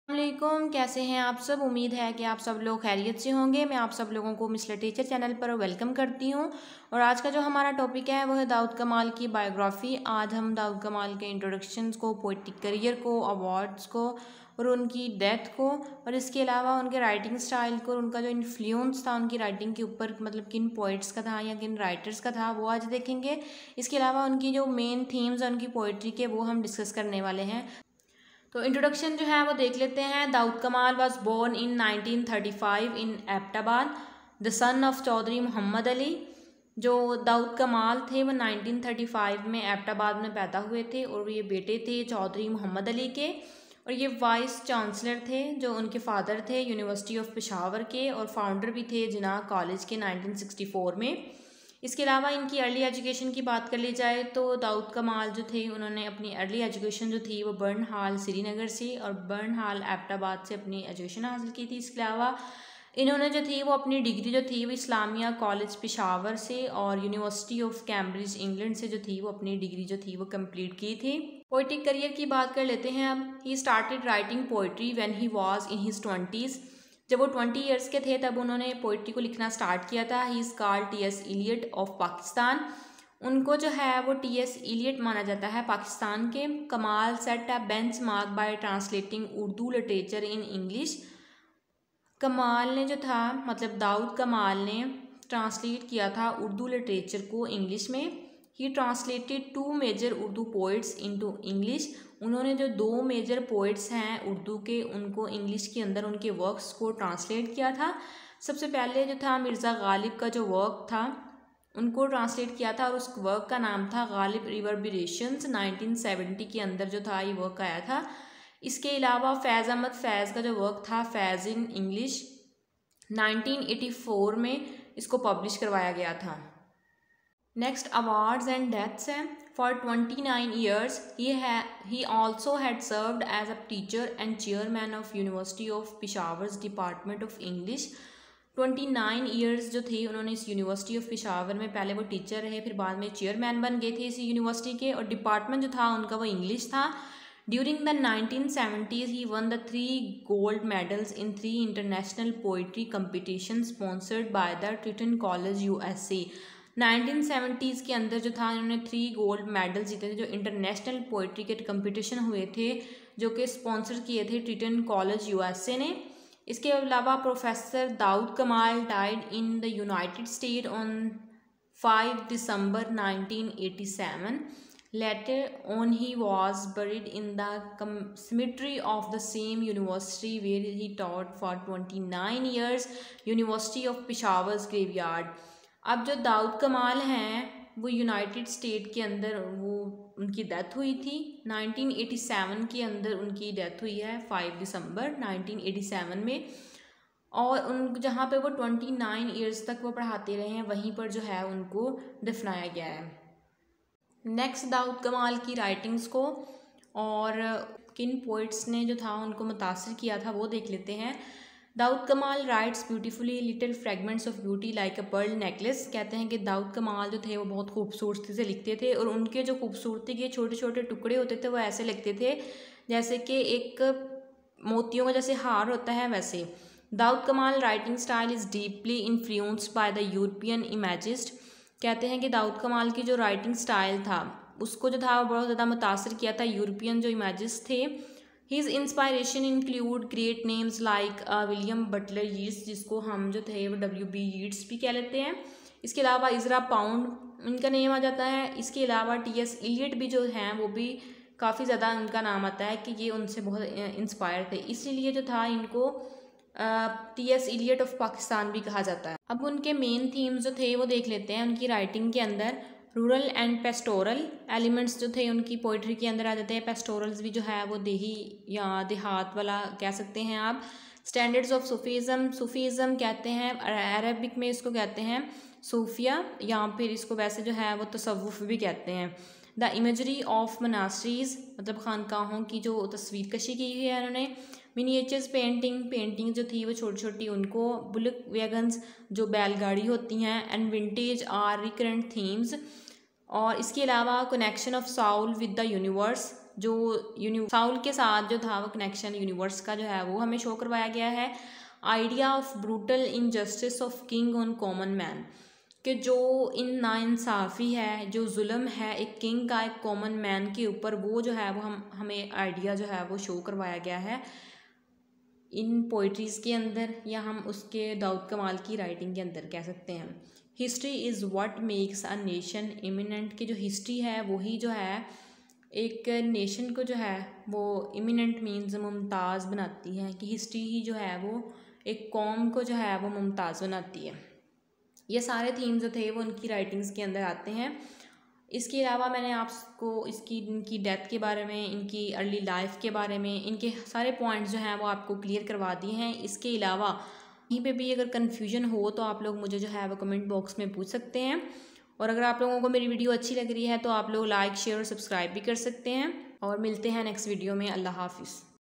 Daud kamal || Biography explained in urdu /Hindi || pakistani famous poet || pakistan literature
Daud Kamal (4 January 1935 - 5 December 1987) (Urdu: داؤد کمال)) was a Pakistani poet who wrote most of his work in the Enligsh language .
His poetry was influenced by modernist English-language poets like Ezra Pound, W.B. Yeats and T.S. Eliot.